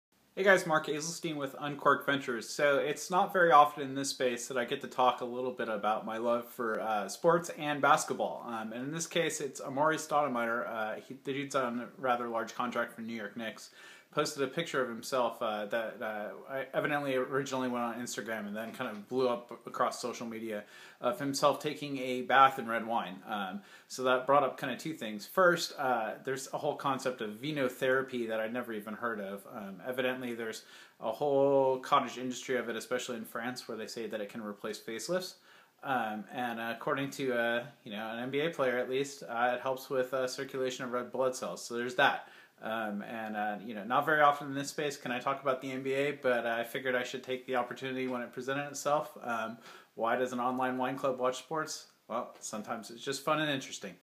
Amare Stoudemire and his red wine bath
I couldn't miss the opportunity to at least mention sports in this space.  Amare Stoudemire was the first major free agent to sign with the Knicks in years and has been trying to figure out how to play with recurrent knee injuries for some time.  His newest therapy, red wine baths.